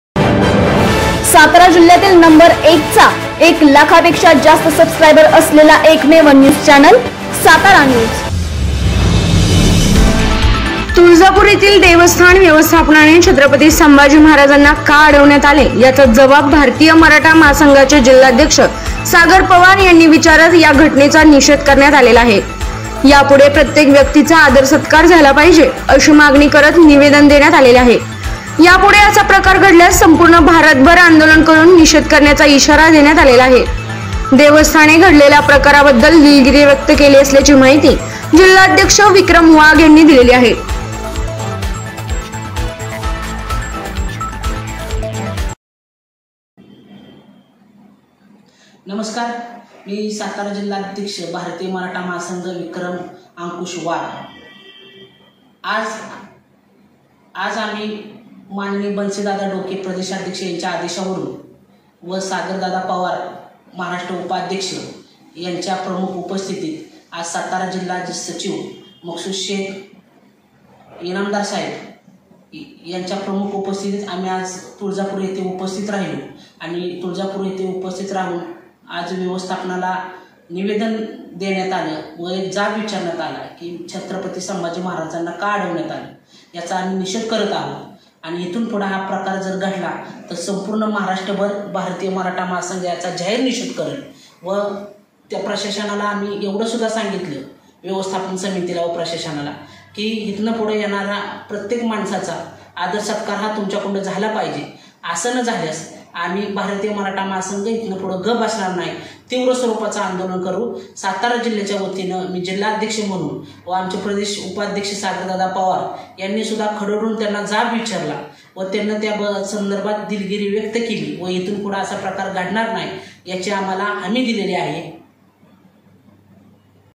छ. संभाजीराजे यांच्याबाबत तुळजापूर येथील घडलेल्या घटनेचा भारतीय मराठा महासंघाने केला निषेध
तुळजापूर